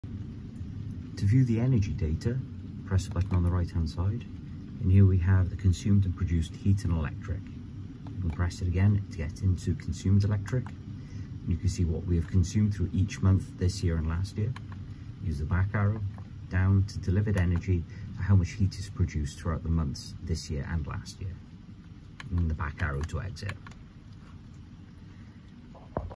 Mitsubishi - Energy Data View
The inbuilt energy monitors only act as a guide based on a few system conditions, for more accurate readings external meters would be required, such as, electric meter to monitor consumption and a heat meter to monitor heat produced.

*adjusting the system without fully understanding it may cause adverse effects such as increased running costs or damage to the system. If in doubt always refer to the user manual or seek expert advise.